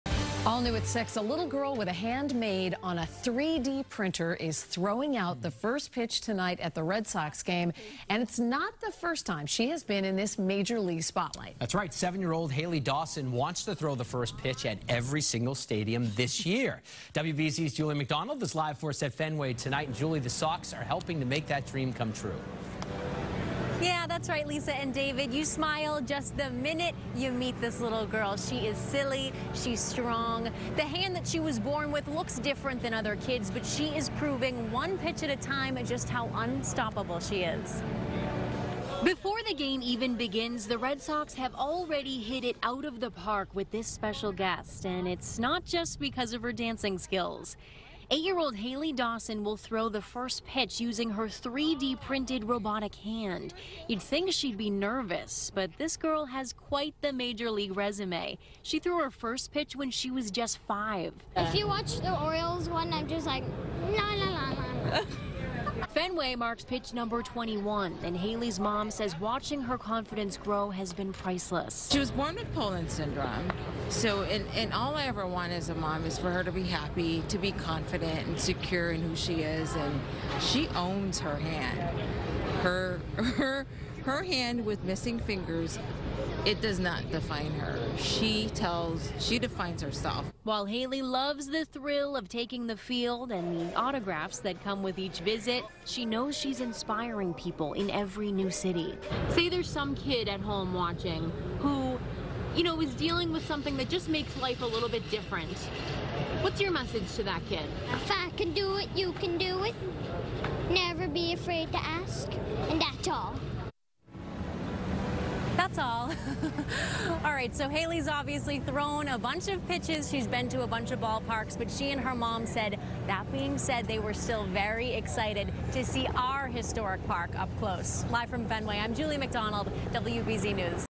Girl With 3D Printed Hand Prepares To Throw Out First Pitch At Fenway Park
Hailey Dawson will throw out the first pitch, using her 3D printed robotic hand. WBZ-TV's Juli McDonald reports.